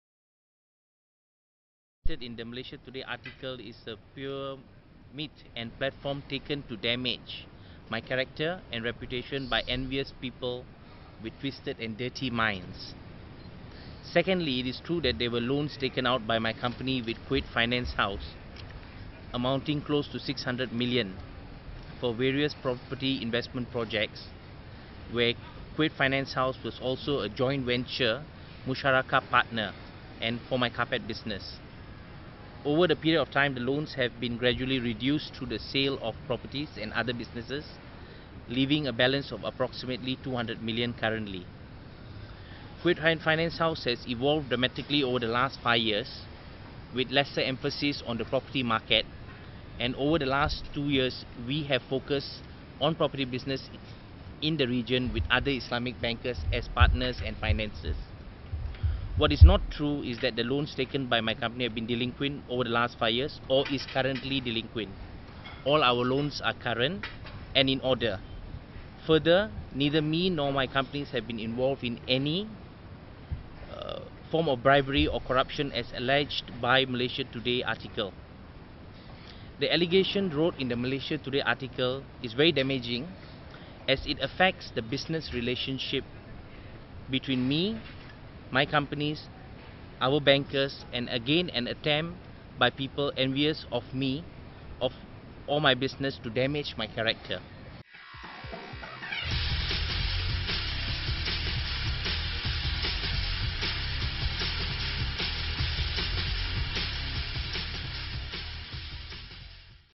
'Carpet man' concedes taking RM600 mil loan
Deepak Jaikishan, the much talked-about 'carpet man' allegedly closely associated with the Prime Minister's wife Rosmah Mansor, today came out in the open to deny claims by blogger Raja Petra Kamaruddin that he is in trouble over a loan scandal with a Middle Eastern bank.